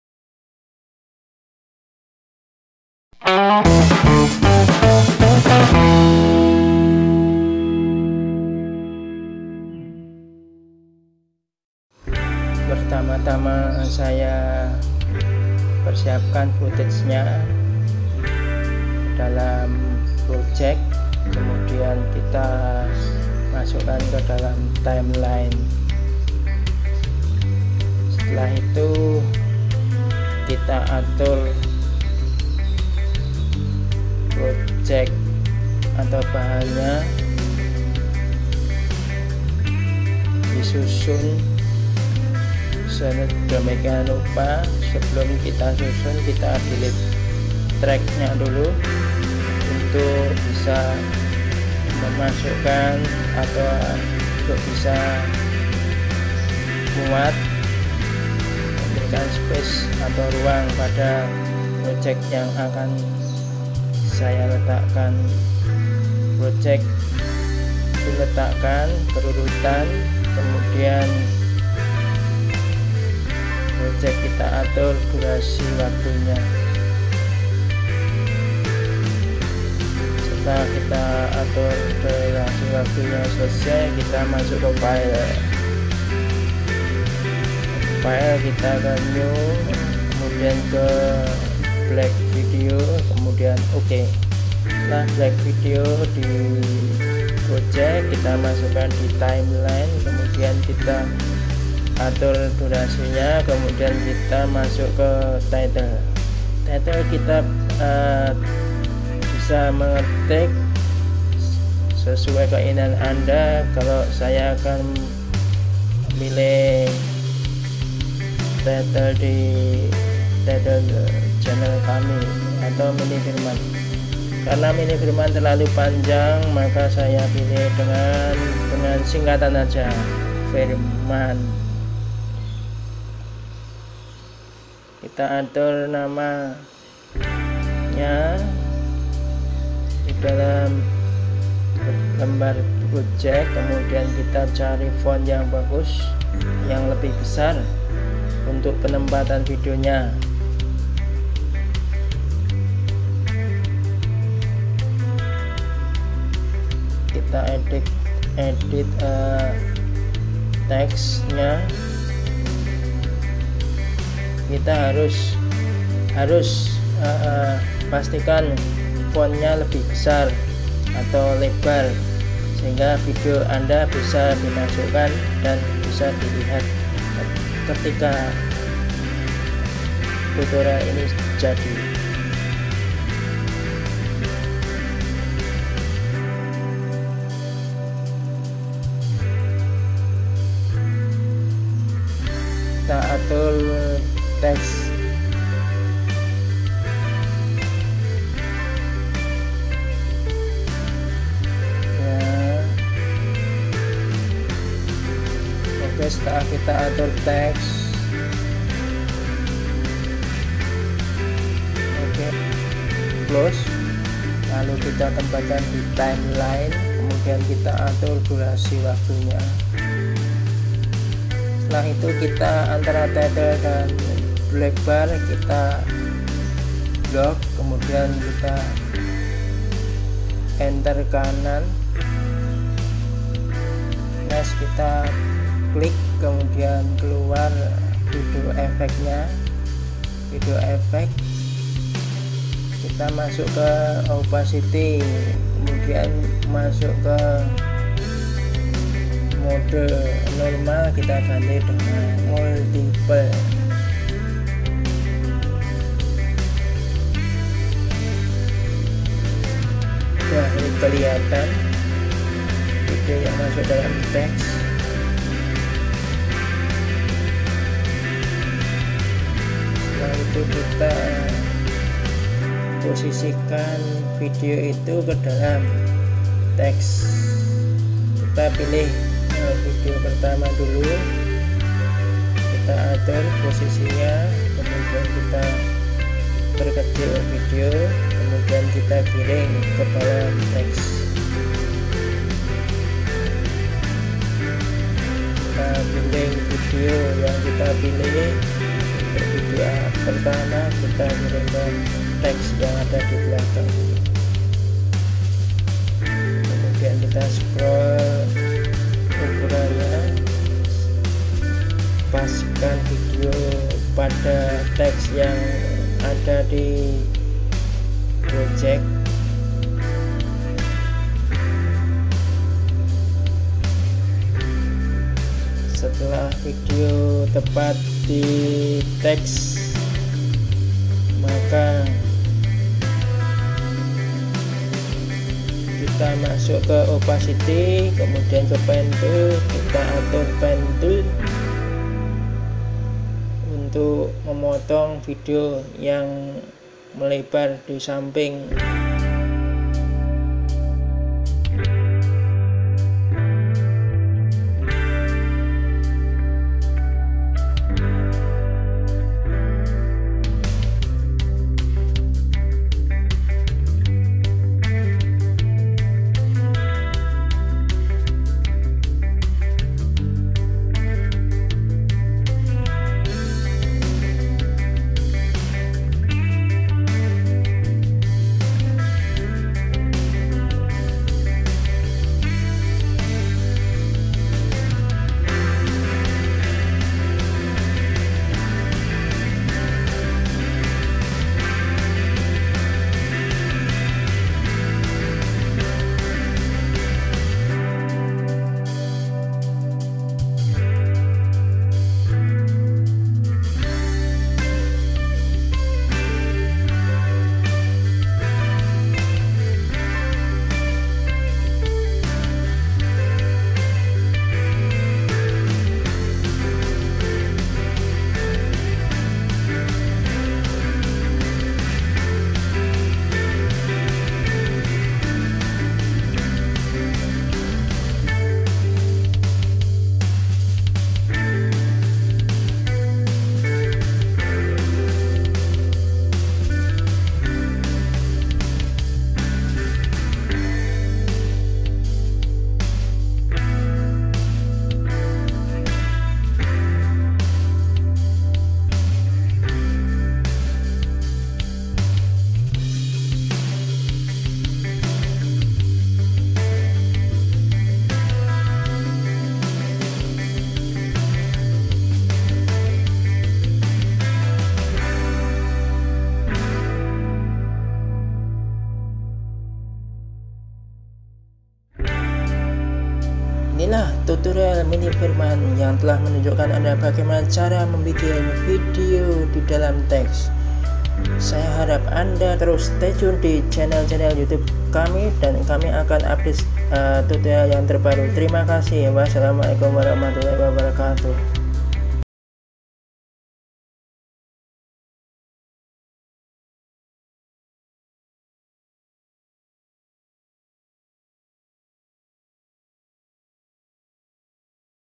How to Place Video Inside Text | Adobe Premiere Pro CC Tutorial
Cara bagaimana memulai Editing place video ke dalam Text menggunakan Adobe Premiere Pro CC